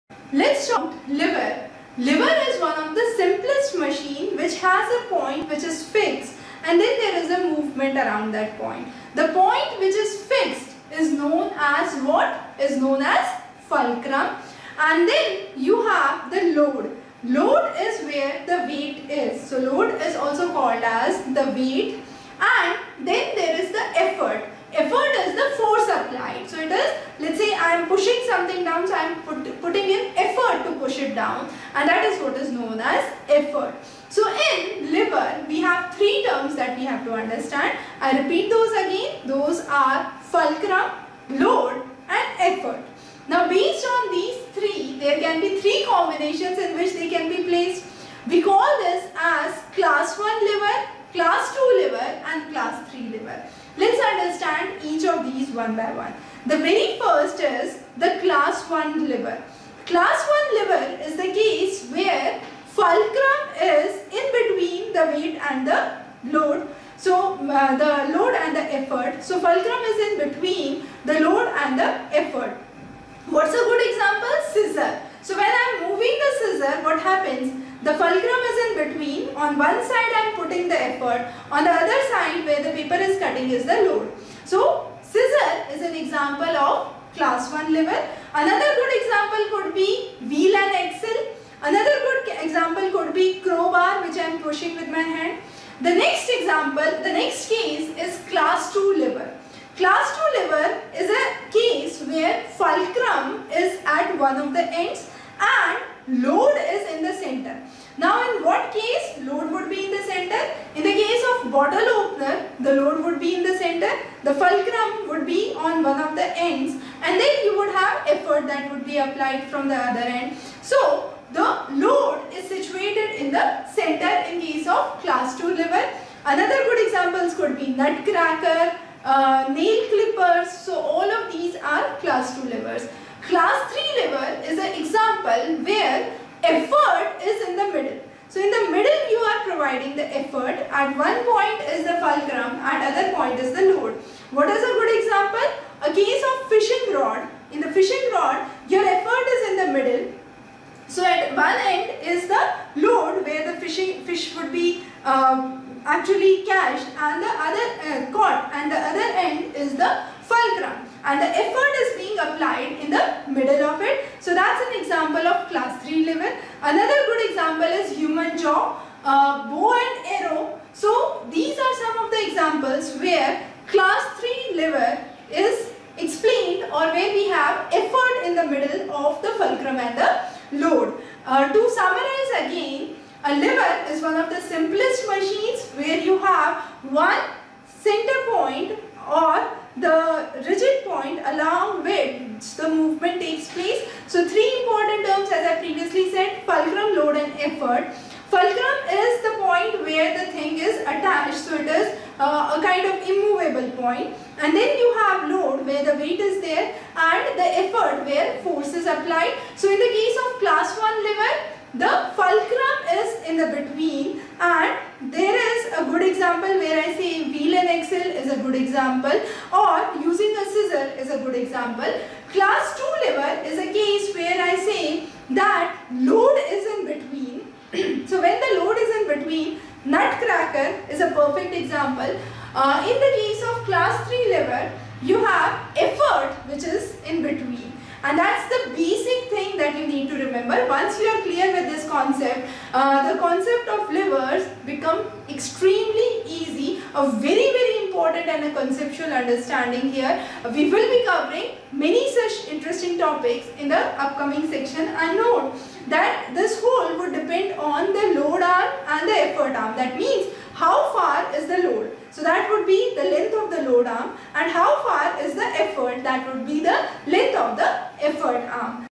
Class 1, Class 2 and Class 3 Lever - Simple Machines | Class 6 ICSE Science | NSO | Olympiad
simple machine lever , level 1 lever , level 2 lever , level 3 lever , types of lever,